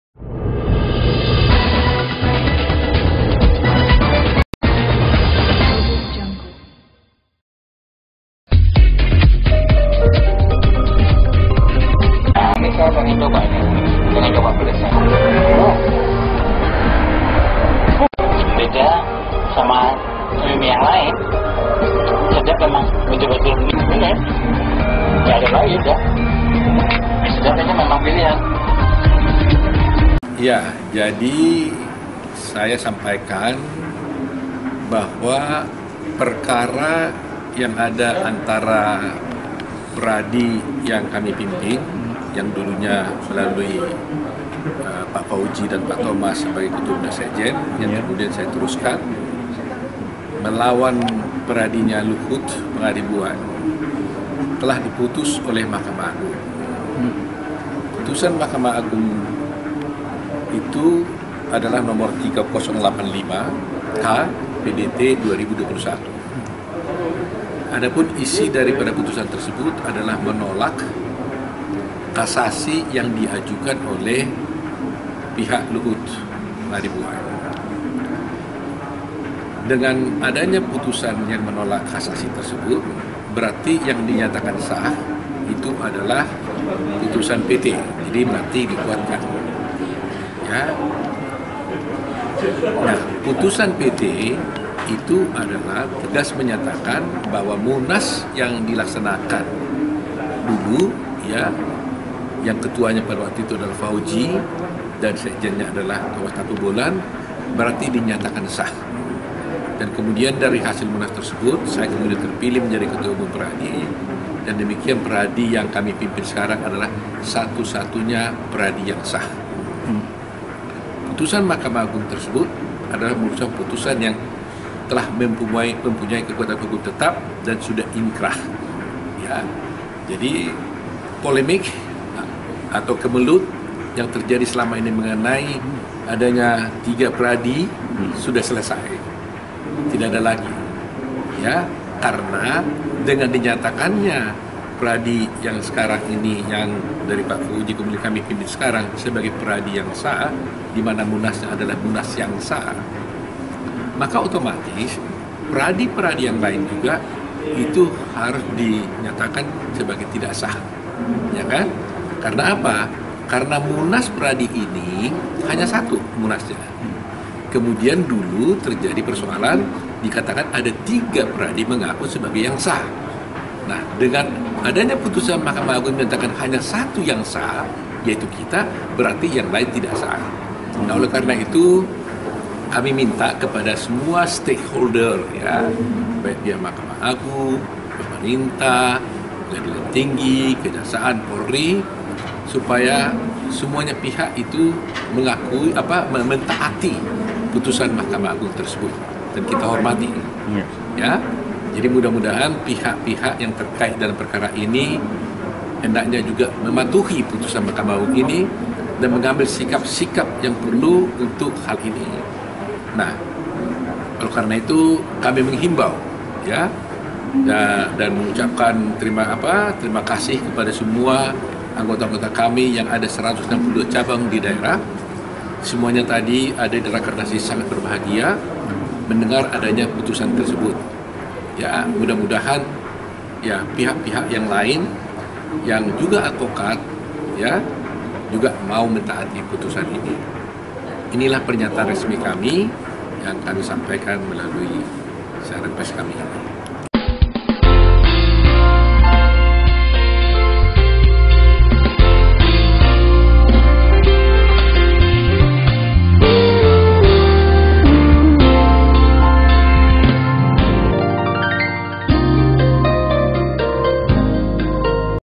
Terkait Putusan Mahkamah Agung (MA) Gonjang Ganjing Peradi Dinyatakan Selesai
Surabaya, The Royal Indonesia TV --  Terkait kisruh di tubuh Perhimpinam Advokat Indonesia (Peradi), Ketua, Umum Peradi Prof. Dr. Otto Hasibuan, S.H., M.C.L., M.M menyatakan, bahwa perkara tersebut telah di putuskan oleh Mahkamah Agung (MA)

"Bahwa perkara yang ada di Peradi yang kami pimpin melalui pak Fauzi melawan Luhut Pangaribuan telah diputuskan oleh Mahkamah Agung," terang Otto pada wartawan Royal TV di hari kedua Rakernas seluruh DPC Peradi , Sangrilla Hotel Surabaya, Jumat (12/11).

Putusan MA tersebut nomer 3085 K/PDT/ 2021. Isinya lanjut Otto menolak kasasi yang diajukan oleh pihak Luhut Pangaribuan. "Maka dengan adanya putusan yang menolak itu, berarti yang dinyatakan sah adalah putusan PT (Pengadilan Tinggi)." ungkapnya

Putusan PT, menurut Otto tegas menyatakan bahwa Munas yang dulu dilaksanakan saat ketuanya Fauzi berarti dinyatakan sah, "Dari Munas kemudian saya terpilih jadi ketua Peradi jadi, Peradi yang saya pimpin saat ini satu satunya Peradi yang sah." tegasnya.

Putusan MA tersebut, sambung Otto telah mempunyai ketetapan hukum tetap dan telah inkrah. Terkait polemik atau kemelut tiga Peradi yang selama menggelinding di publik bagi dia sudah selesai.

"Maka otomatis Peradi yang lain harus dinyatakan tidak sah, karena Munas Peradi hanya satu Munasnya," ujar dia.

Persoalan adanya 3 Peradi yang sama sama mengaku sah baginya sudah tidak berlaku.

"Jadi dengan adanya putusan MA yang menyatakan satu yang sah yaitu kita, berarti yang lain tidak sah." seru dia.

Karenanya, pihaknya meminta kepada semua stakeholder baik MA, pemerintah PT, Kejaksaan,  Polri supaya menaati putusan MA dan menghormatinya.

"Jadi mudah mudahan pihak terkait masalah ini hendaknya mematuhi keputusan MA ini." tegasnya.

Ia pun mengucapkan terimakasih kepada semua anggota165 cabang Peradi
di daerah, sembari dia berharap pihak lain dan advokat mentaati keputusan tersebut.

"mudah-mudahan pihak yang lain yang juga advokat juga mentaati putusan ini." tandasnya.